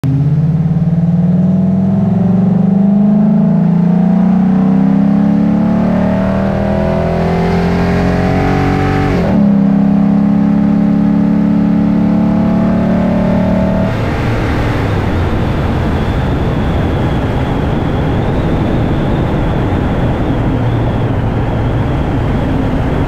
Ford 5 L coyote second run
2015 Ford 5 L second run on map thinking that I might have to add another 200 RPMs to the shift